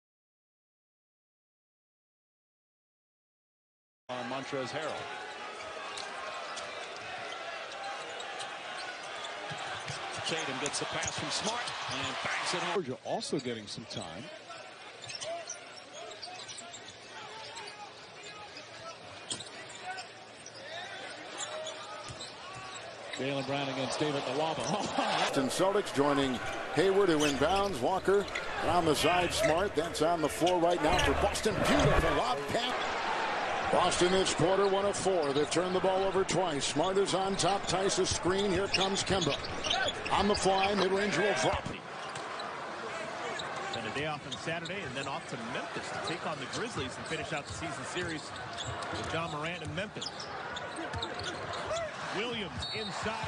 Celtics Shuffle SLOB
Shuffle-Stagger or Shuffle-Down sets from a sideline inbounds. Brad Stevens 2020 Boston Celtics playbook